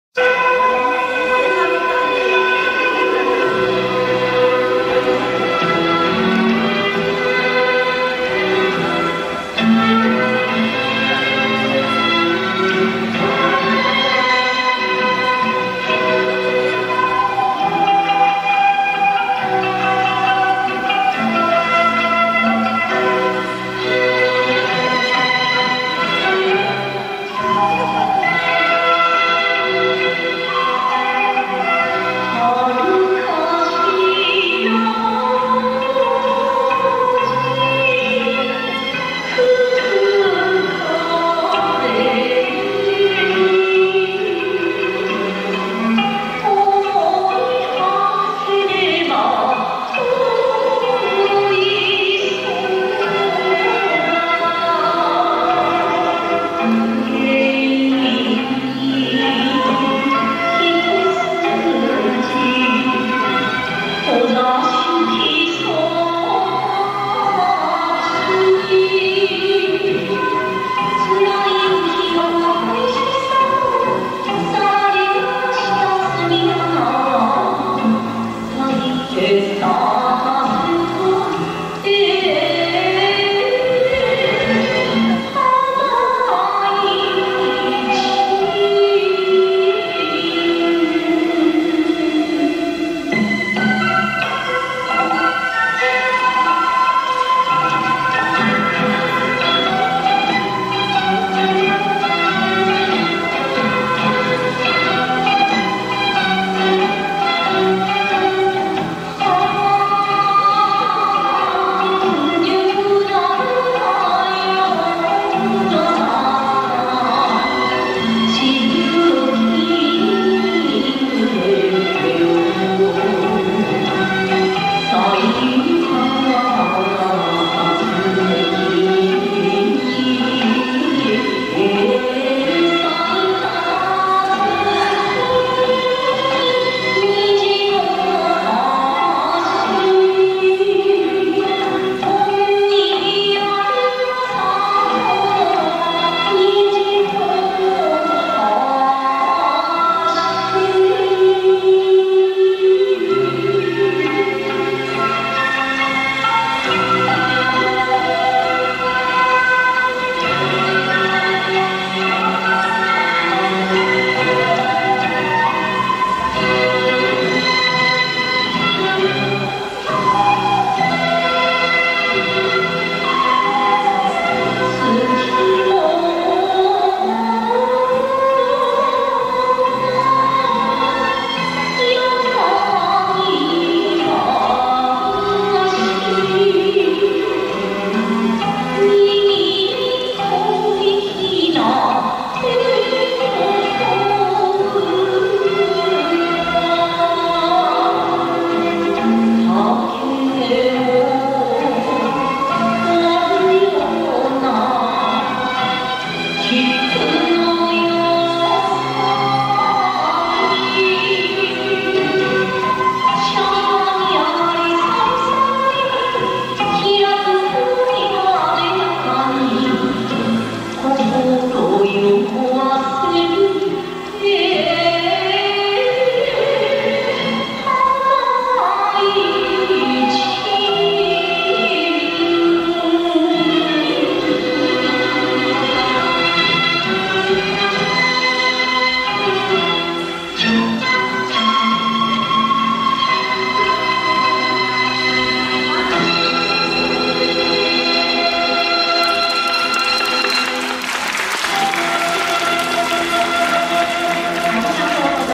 2019 江戸川区民謡舞踊大会１７
江戸川区総合文化センターの、第６２回秋季「江戸川区民謡舞踊大会」から。